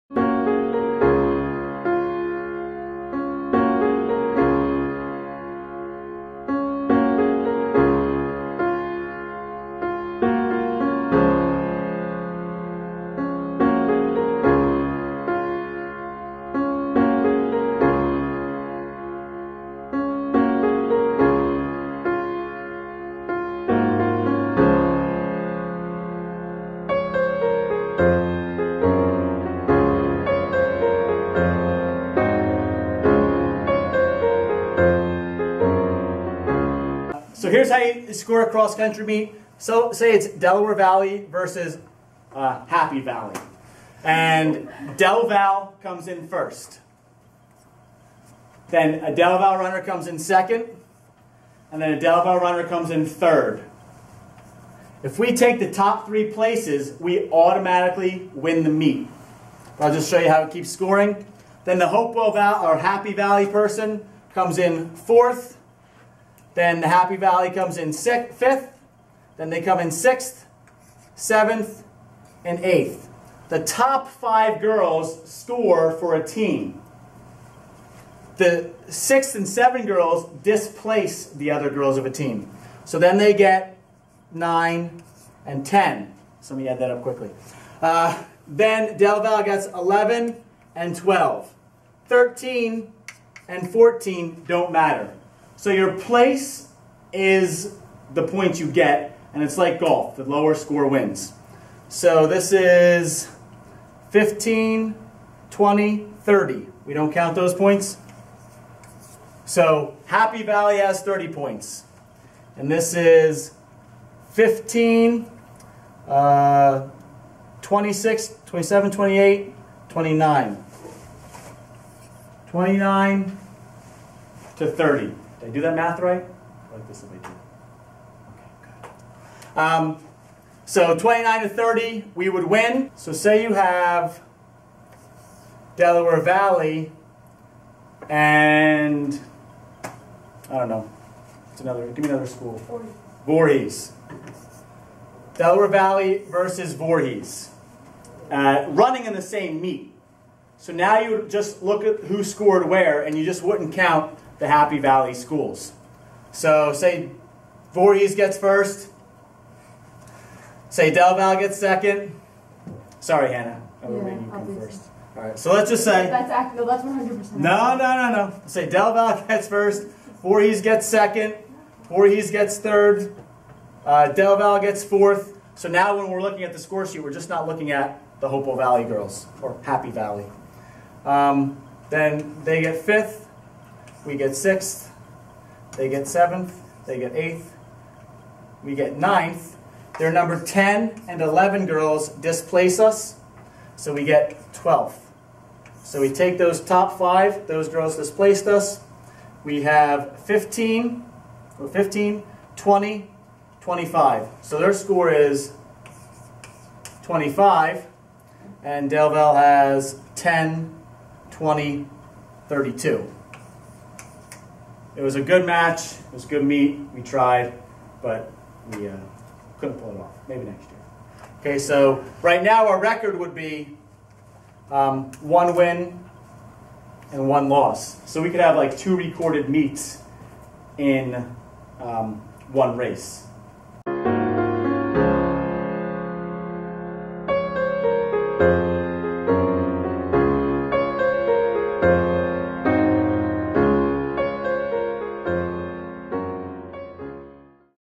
Cross Country Scoring, How to Score, a XC or Cross Country, Dual Meet
Learn how to score a cross country dual meet in the short, easy to follow, video. Scoring a cross country, or XC, dual meet. How is a cross country dual meet scored?